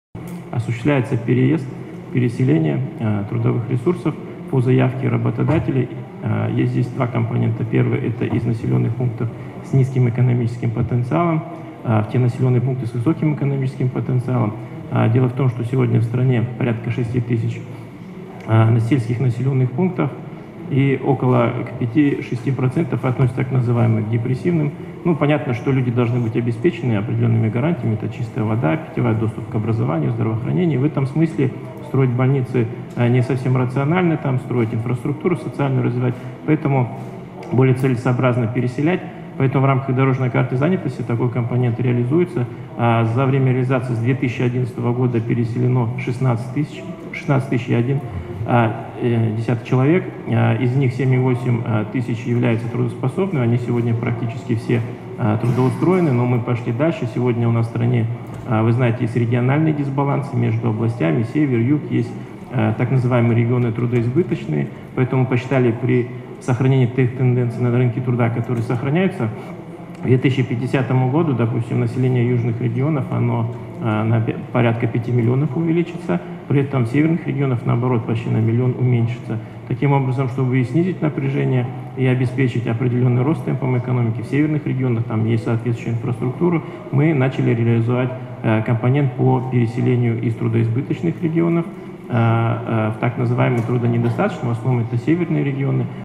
Комментарий Д. Аргандыкова
Вице-министр здравоохранения и социального развития Даулет

Аргандыков рассказал о предварительных итогах программы «ДКЗ – 2020».